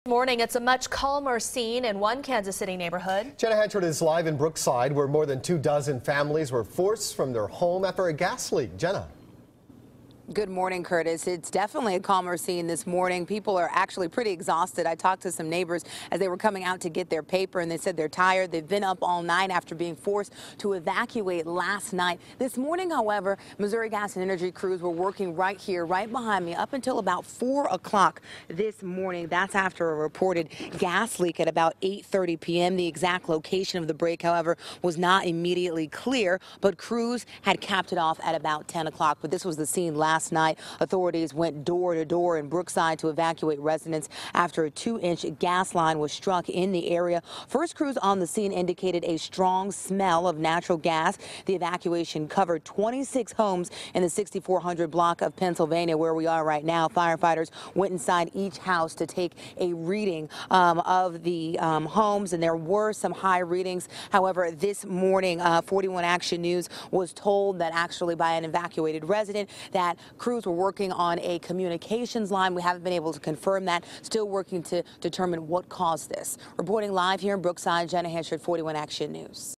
Gas leak forces evacuation of KC neighborhood
Authorities went going door-to-door Wednesday night in Brookside to evacuate residents after a two-inch gas line was struck in the area.